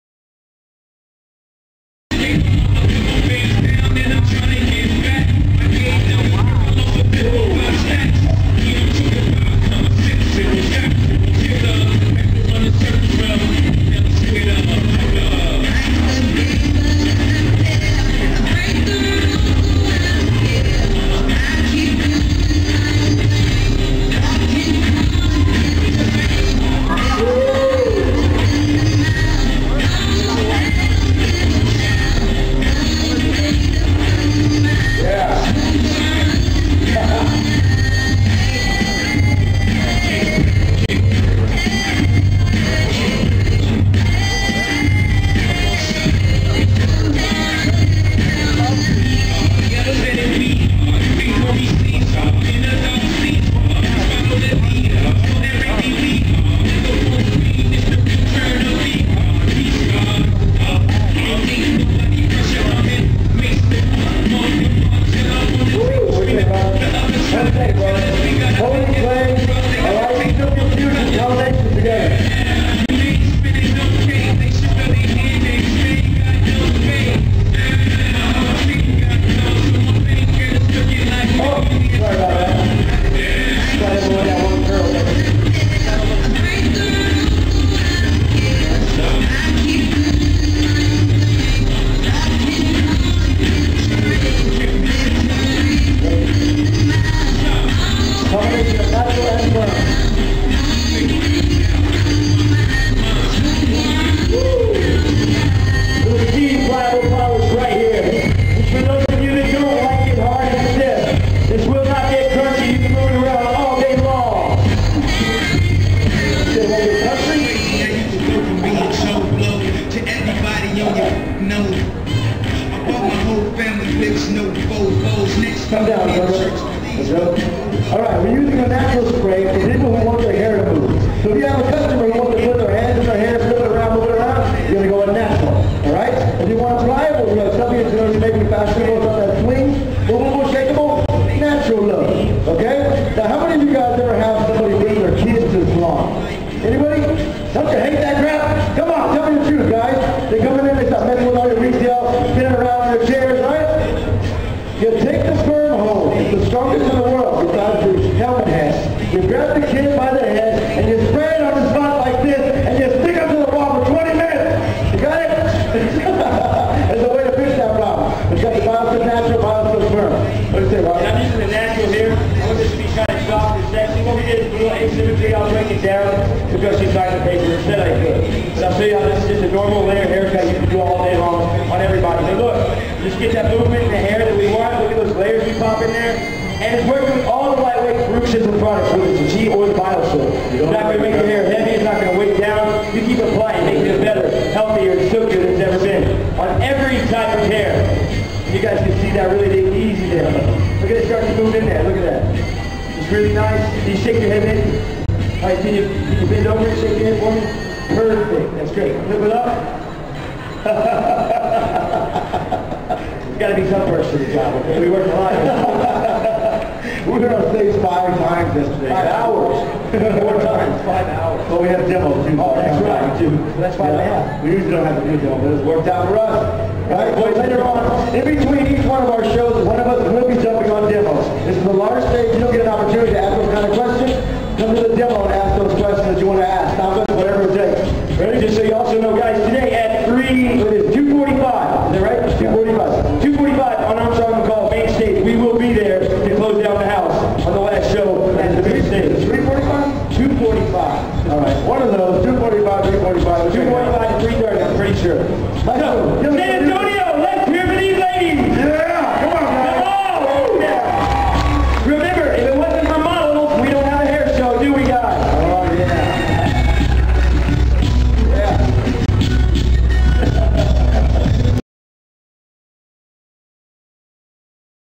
High Energy CHI Platform Artists: Joe & Rocky at The World's Fair of Hair & Cosmetics 2009
Witness the amazing energy of CHI platform artists as they perform live at the World's Fair of Hair & Cosmetics held in San Antonio Texas. Follow along as they share tips and tricks to turning anyones hair into a work of art!